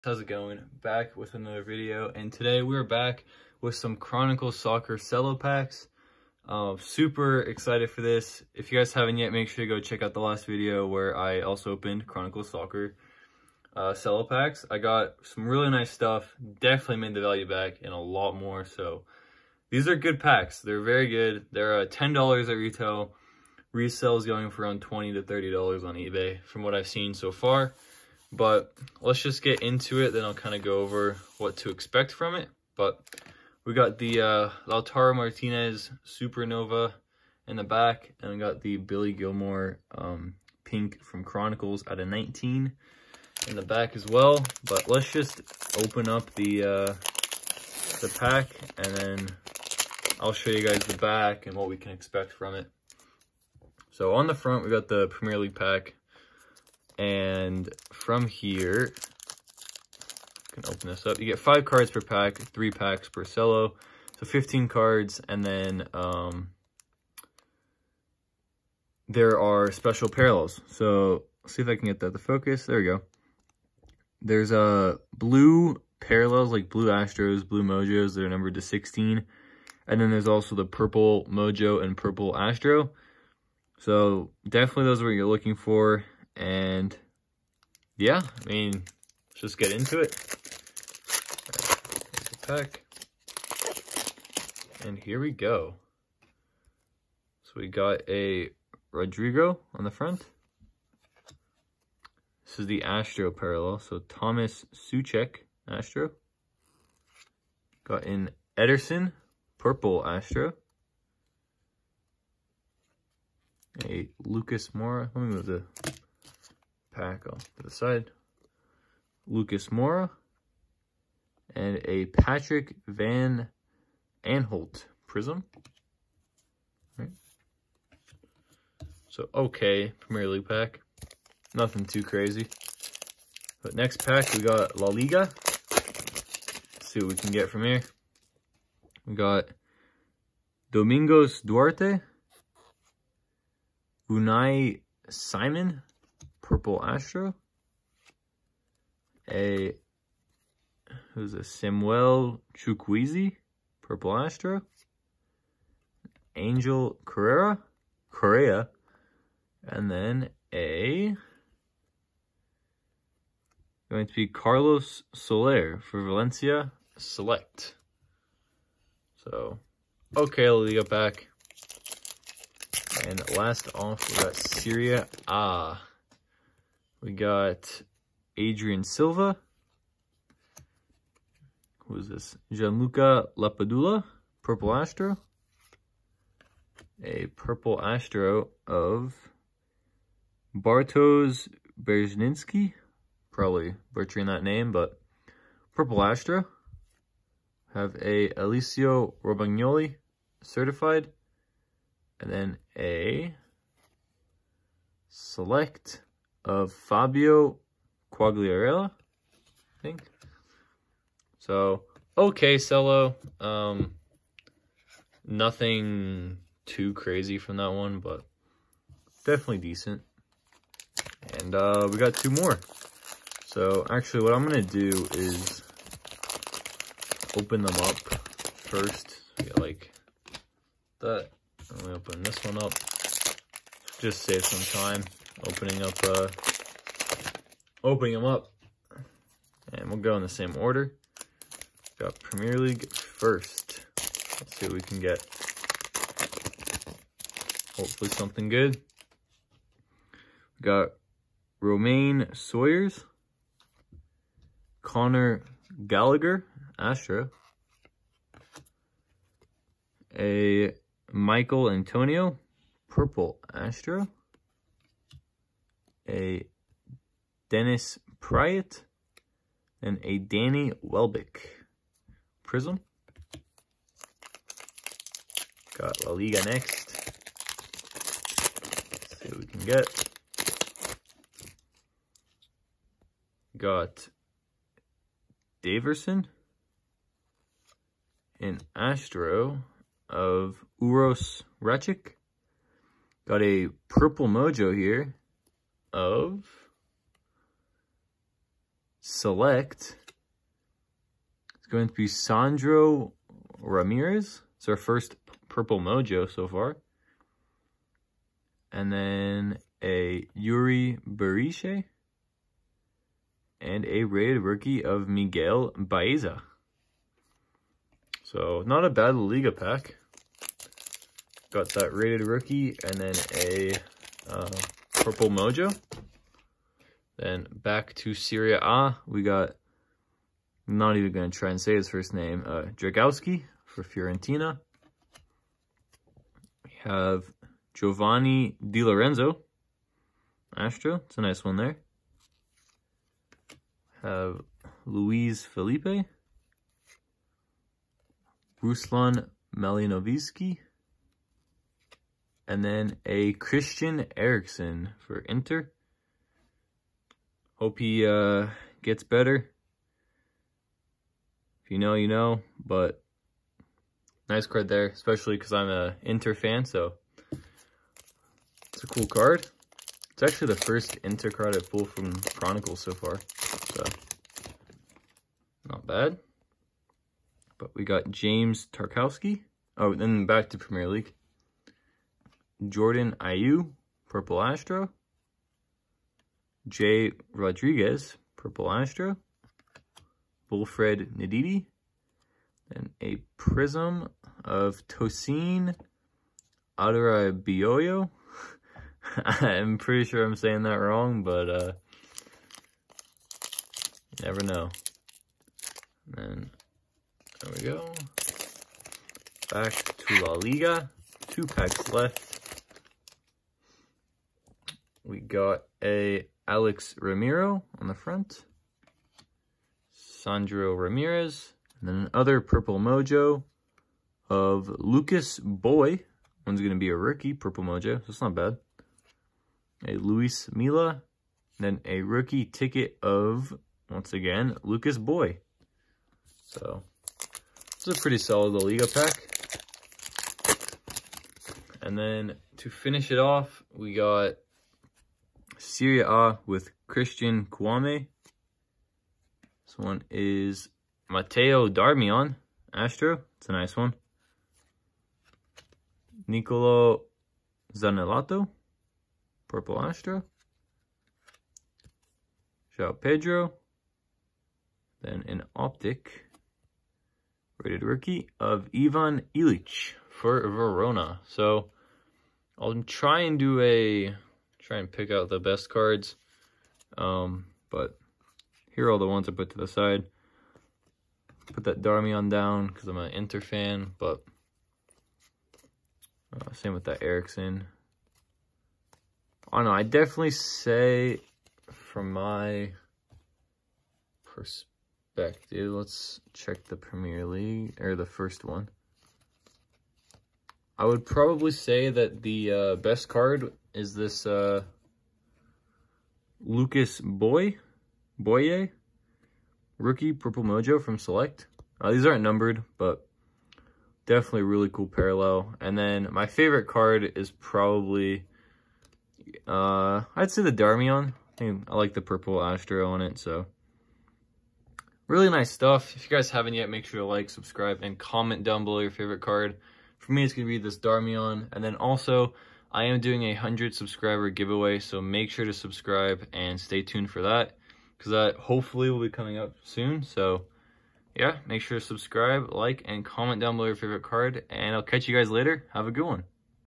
2020-21 Chronicles Soccer Cello Packs!
Today I opened 3 chronicles cello packs!